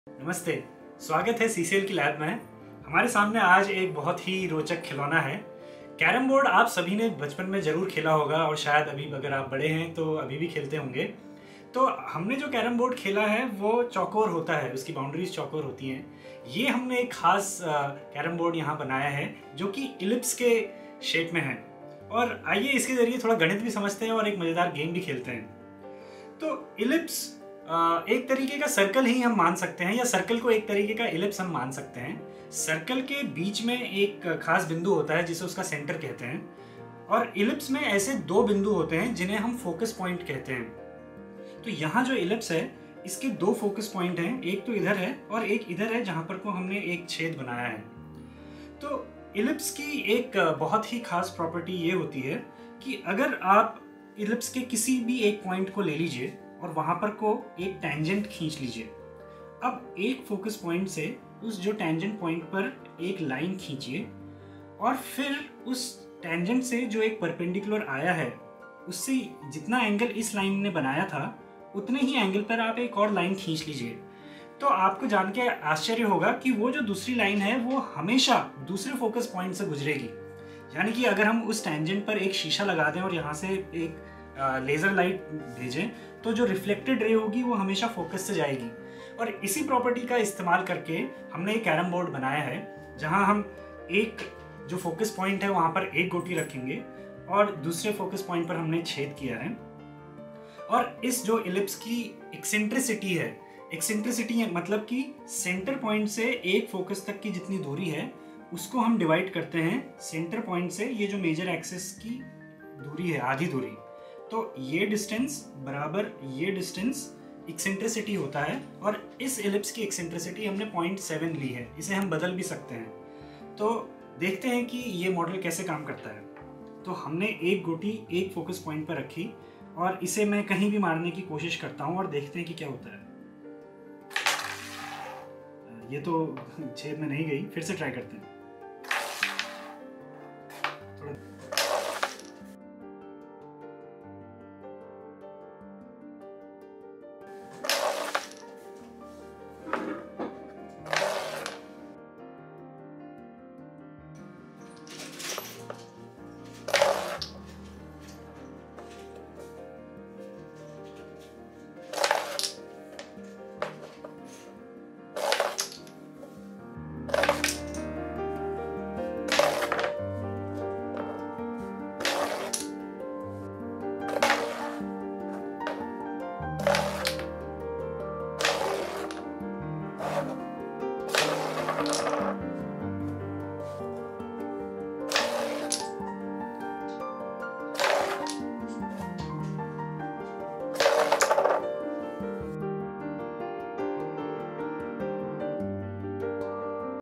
Magical Mathematical Carrom board
You must have played carrom board. Would you like to be a master player by putting the discs in the hole with every shot?

For a circle the distance of a point on the circle is constant from the center. An Ellipse has two 'centers' called focus points. The  sum of distance of a point on the ellipse from both the focus points is constant.

At CCL we have made a new carrom board which is an ellipse. It is based on the unique reflection property of an ellipse that a ray emanating from one focus and incident on any point on the ellipse will reflect back and pass through the other focus point.

About Us

Say hello on Facebook: goo.gl/f3a1Yo